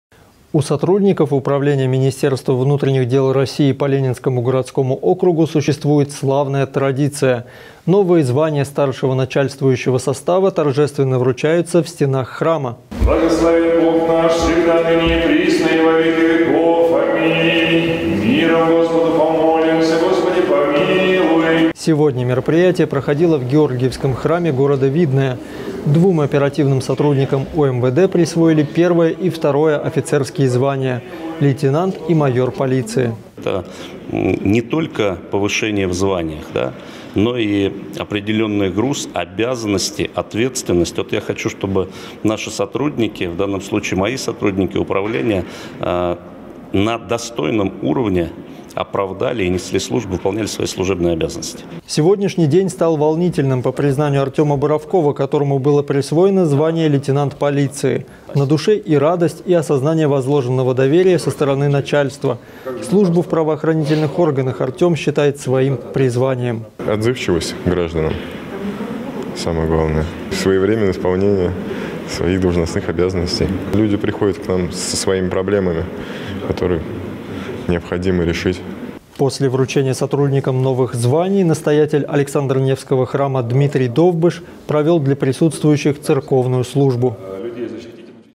«Новые звания» - торжественное присвоение очередных званий сотрудникам УМВД
У сотрудников управления министерства внутренних дел России по Ленинскому городскому округу существует славная традиция. Новые звания старшего начальствующего состава торжественно вручаются в стенах храма.